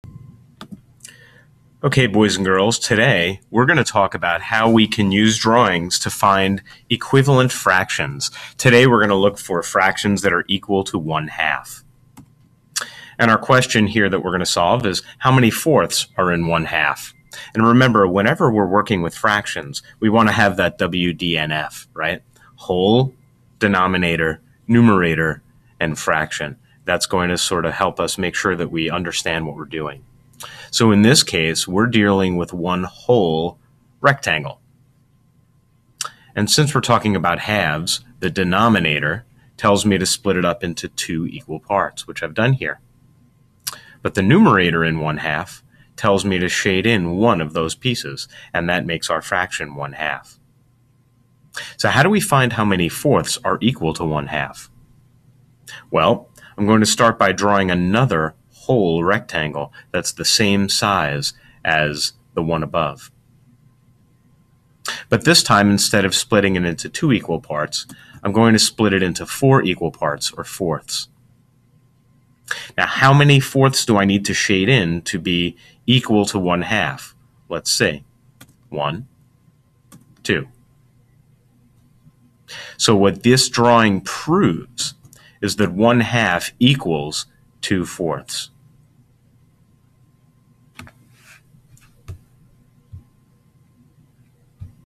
Fractions Tutorial: Equivalent Fractions (Fourths and Halves)
This video demonstrates how to use a bar model to determine how many fourths are equal to one half.